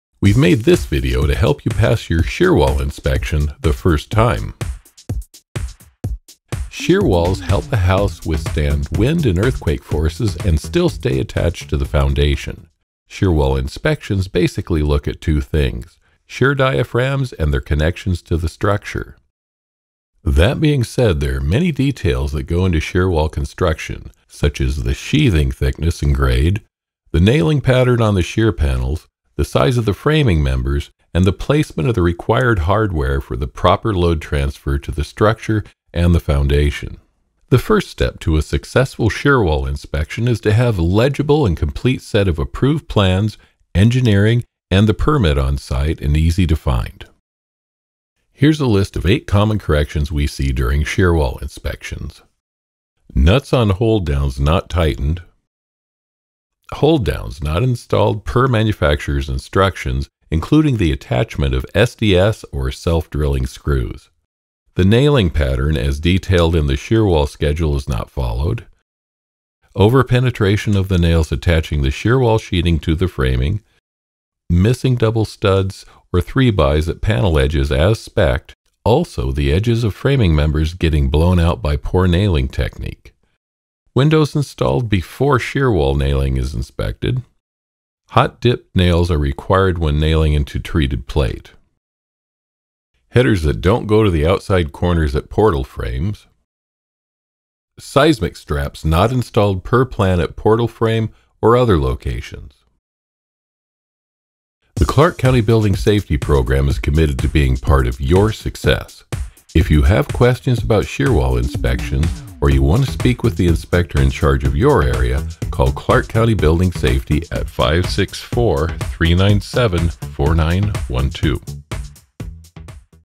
Shear Walls Video v4
Learn about shear walls through Clark County, Washington Building Safety